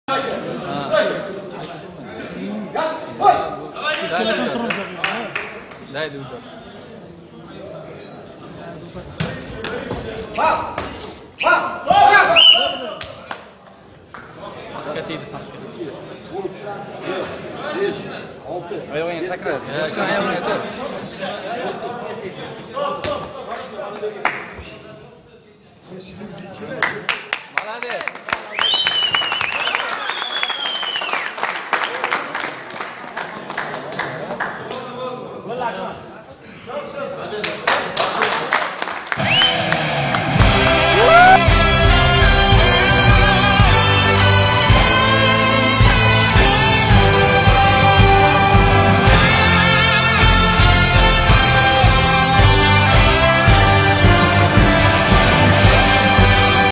Nokaud 15 Qo'l jangi chempionati Xudoynazarov Habibullo murabbiyi va akasi Dilshod Xudoynazarov
Nokaud 15 Qo'l jangi chempionati Xudoynazarov Habibullo murabbiyi va akasi Dilshod Xudoynazarov DFC fight sport